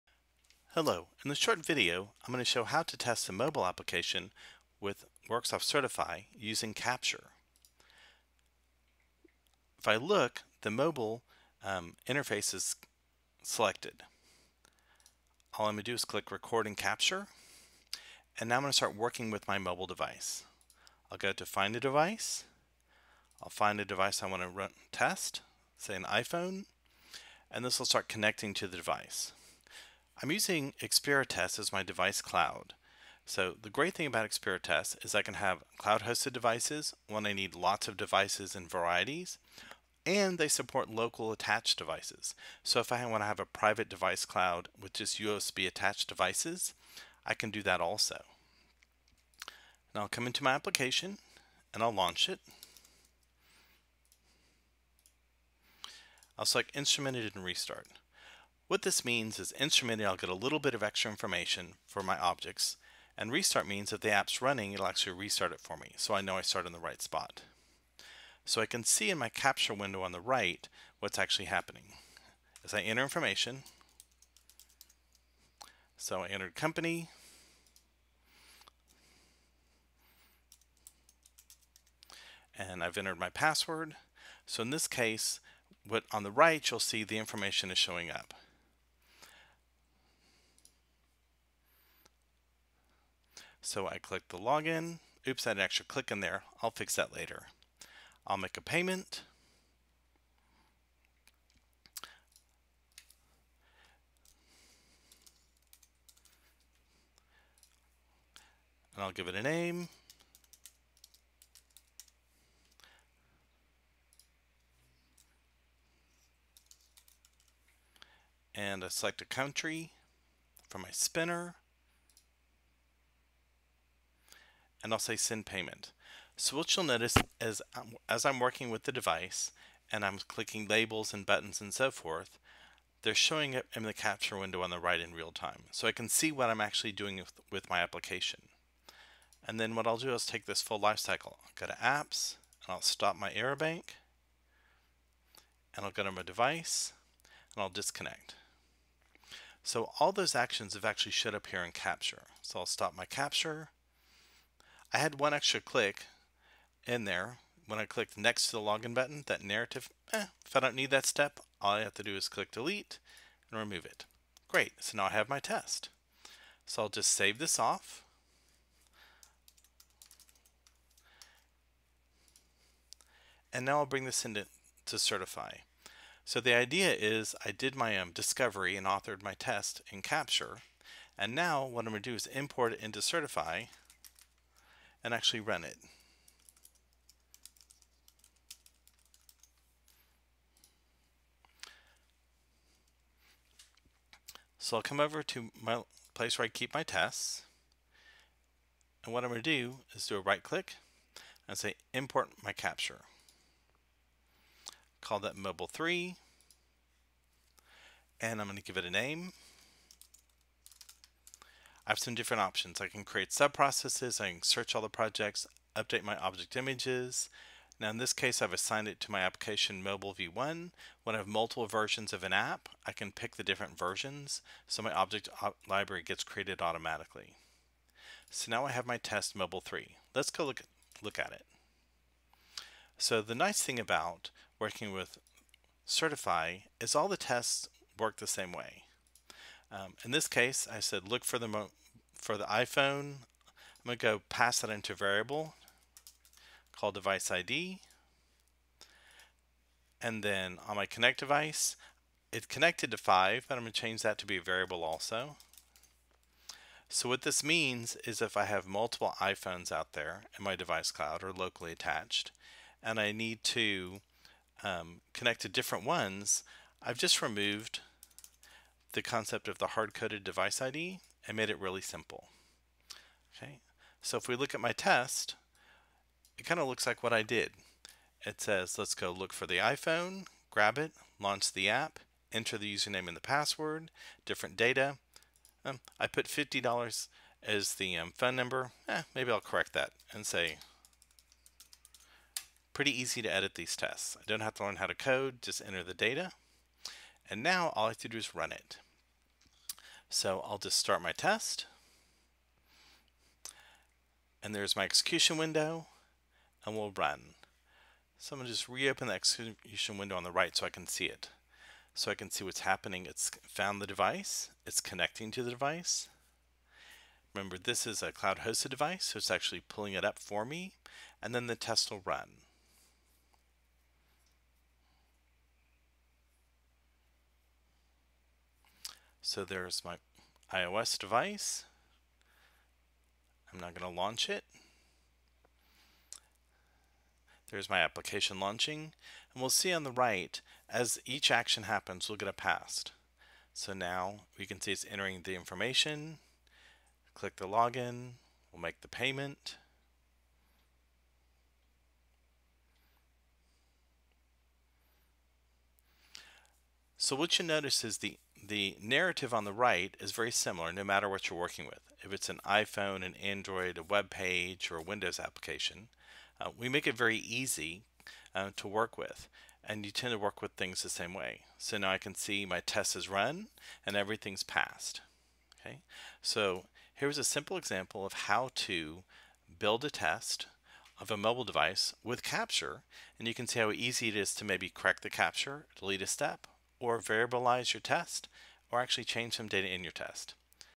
Mobile Test Creation with Capture 2.0
Learn how quick and easy it is to create, edit and run mobile tests using Capture 2.0 with Worksoft Certify and supporting Experitest integration